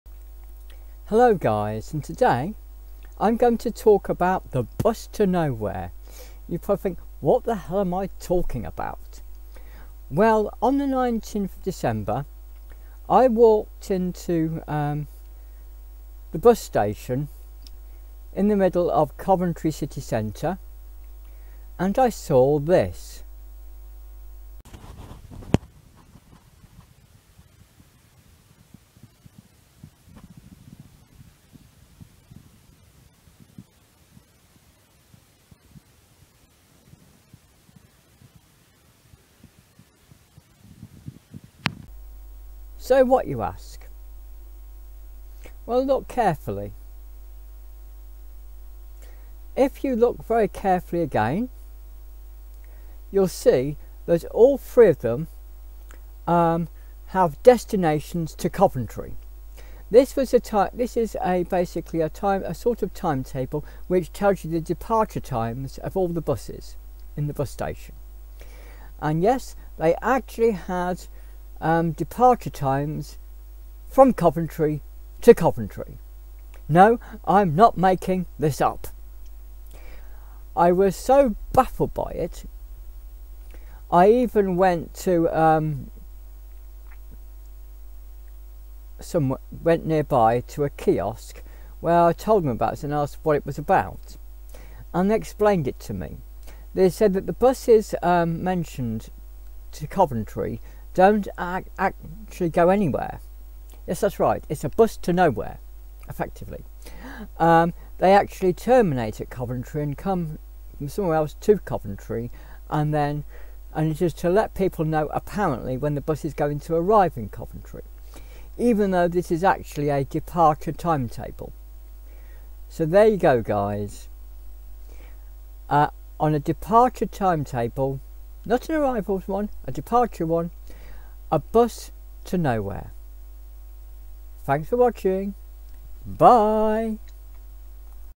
Cliff Discusses - Buses To Nowhere
I was in Pool Meadow Bus Station in the centre of Coventry City Centre when I saw some buses bizarrely going to a strange destinations on the departure timetable.

What could it be about?